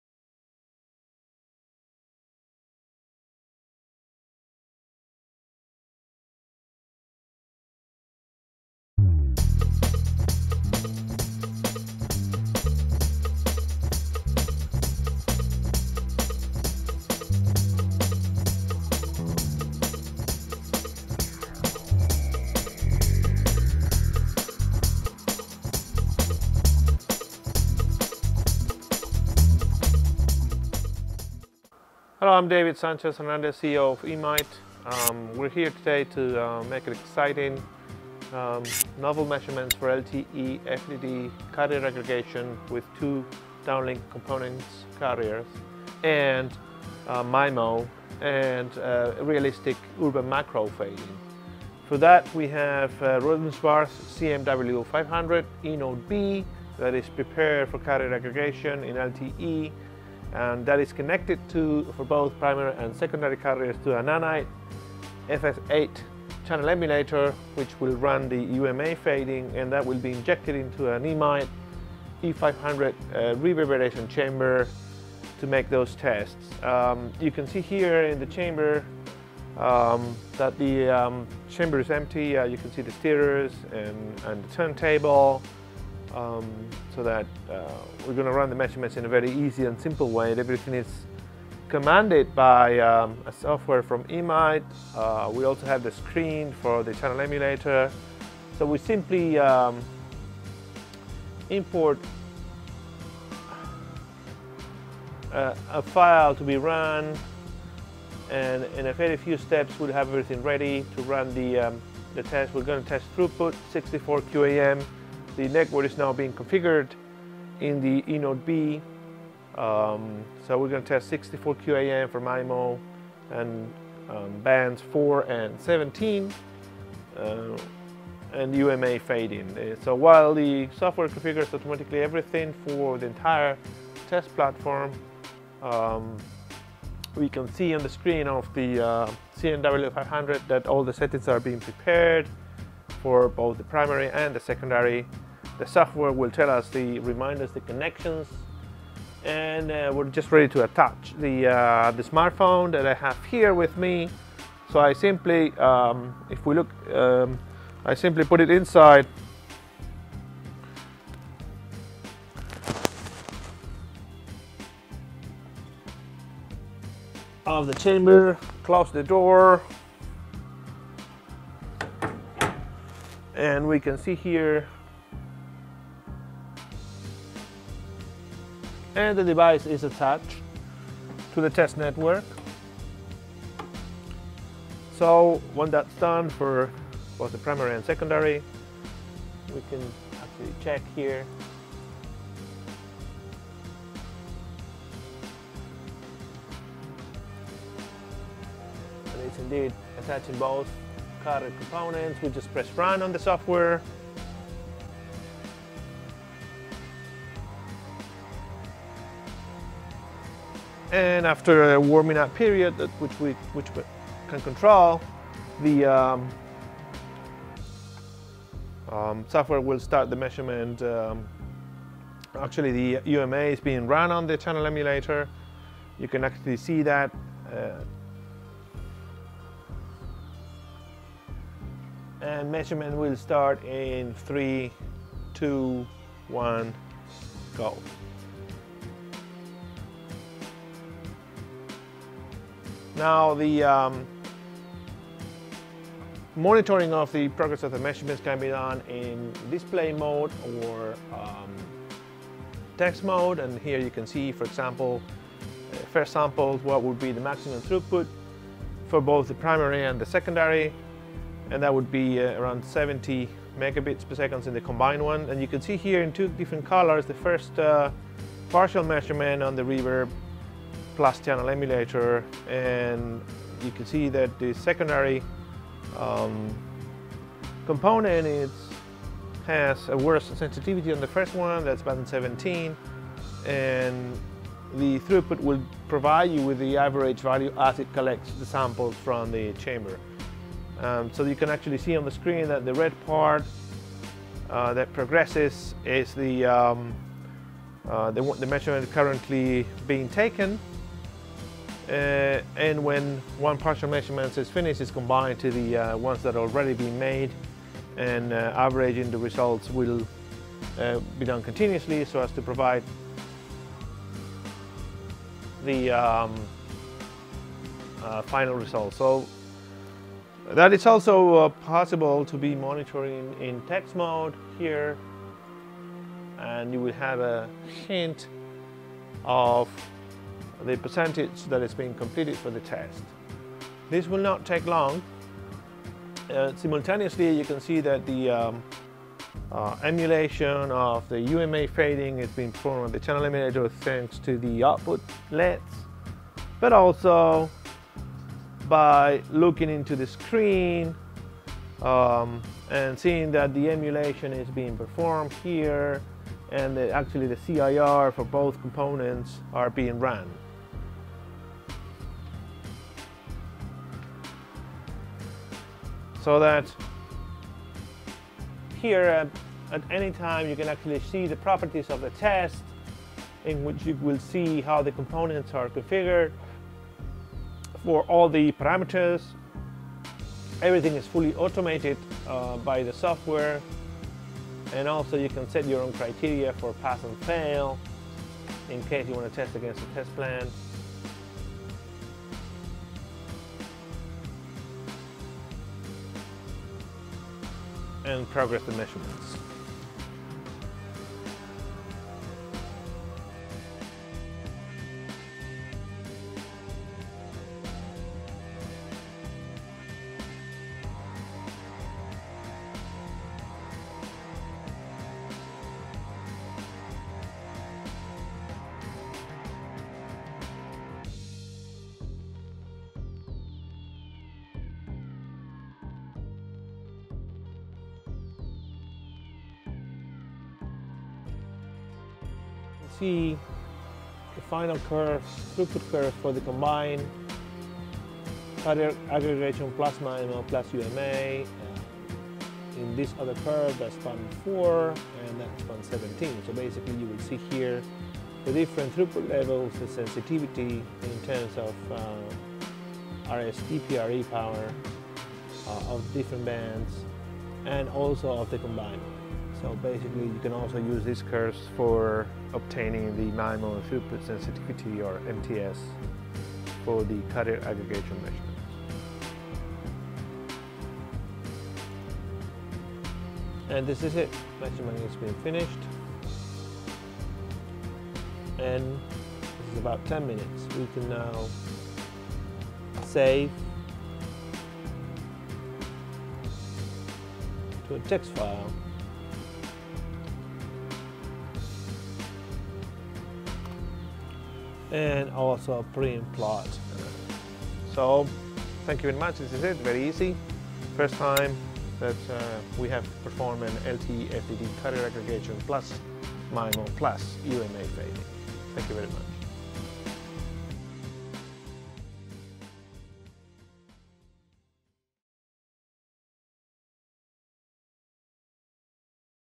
LTE Carrier Aggregation with EMITE E500, Anite Propsim FS8 and R&S CMW500
In this video we make an exciting and novel measurement for LTE FDD Carrier Aggregation with two downlink component carriers + MIMO + realistic Urban Macro fading (UMa).
We use:
      -EMITE E500 (Reverberation Chamber).
      -Anite Propsim FS8 (Channel Emulator).
      -Rohde&Schwarz CMW500 (Base Station Emulator).